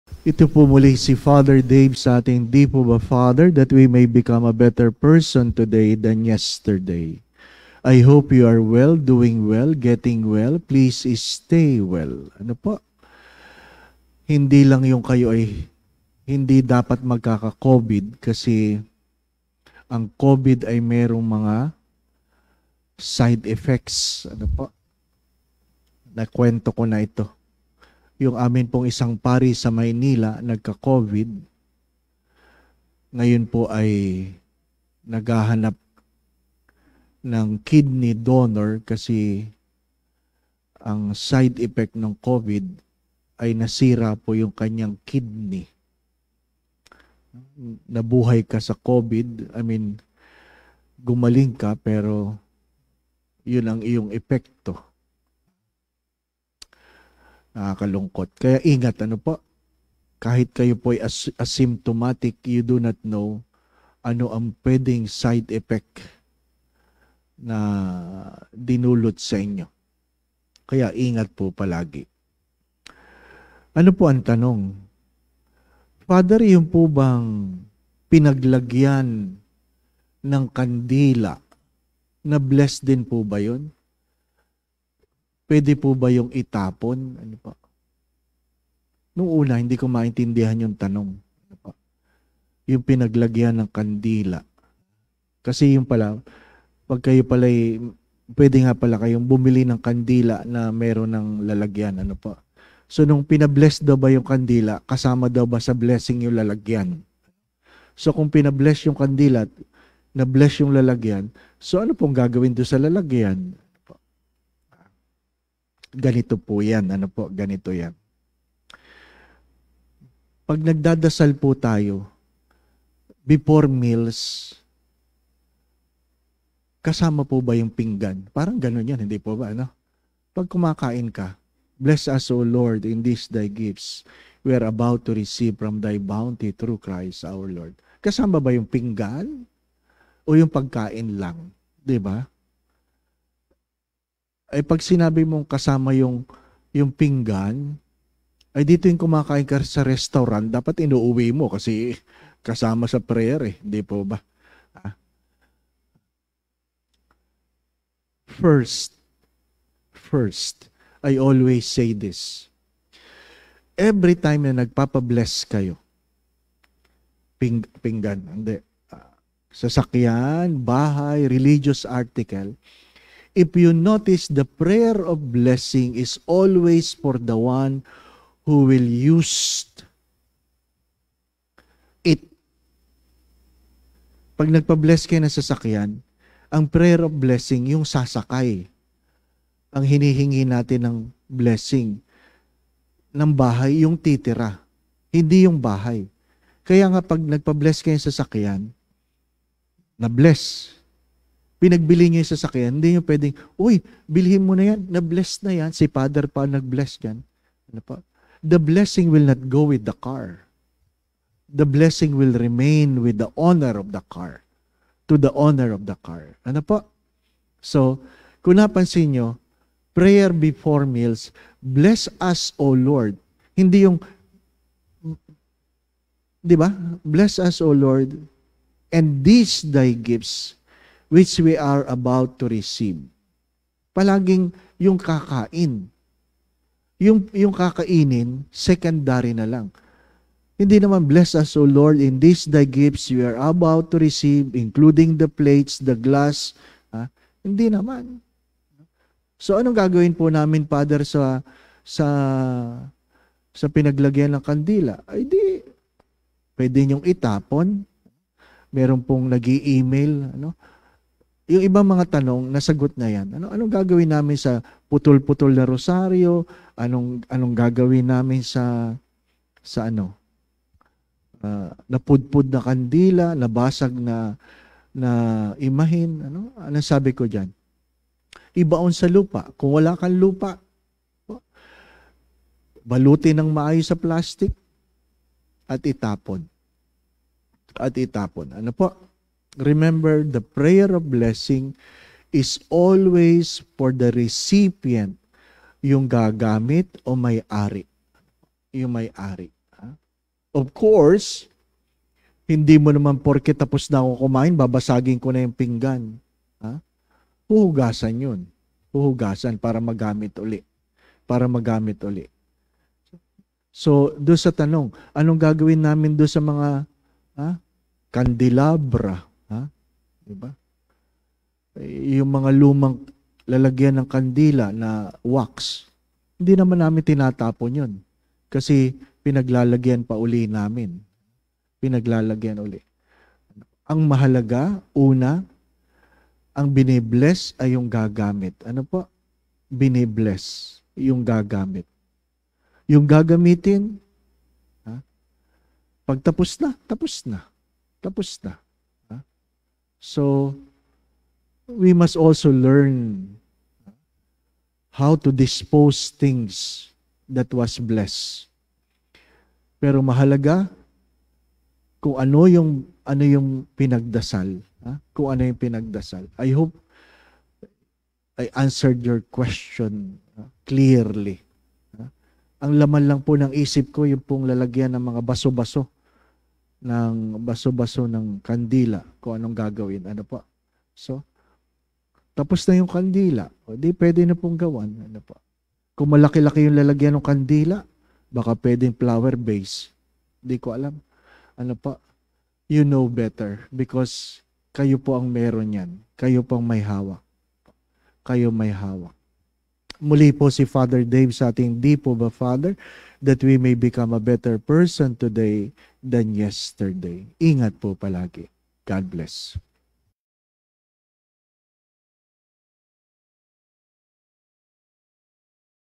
dipobafrdave (Ep. 466)- NA BLESS DIN BA YUNG MGA PINAGLAGYAN NG KANDILA?
Q&A with Fr. Dave Concepcion after the 7:00am weekday mass at Sta. Maria Goretti Parish (Feb. 28, 2022)
online mass schedule:
Monday to Saturday : 7:00am
Sundays : 9am, 10:30am and 5pm